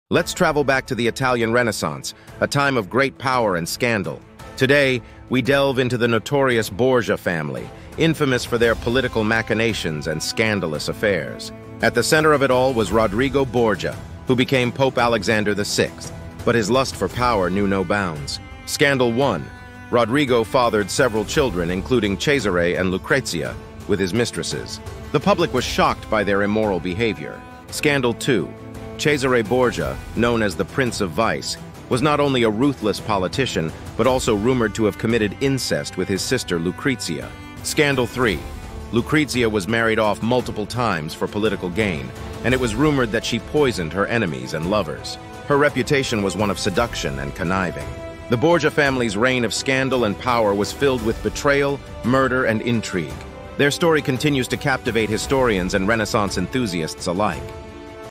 Borgia Scandals: A Tale of Power, Intrigue, and Betrayal | Al2past
Uncover the riveting saga of the notorious Borgia family, a Renaissance dynasty shrouded in scandal, power struggles, and dark secrets. From Pope Alexander VI's controversial reign to the family's intricate web of alliances and betrayals, this journey explores the tumultuous lives of the Borgias. Step into the heart of Renaissance Italy, where ambition and treachery shaped destinies, leaving an indelible mark on history. Join us as we unravel the captivating story of the Borgia family's rise and fall, revealing the scandalous tales that continue to echo through the corridors of time.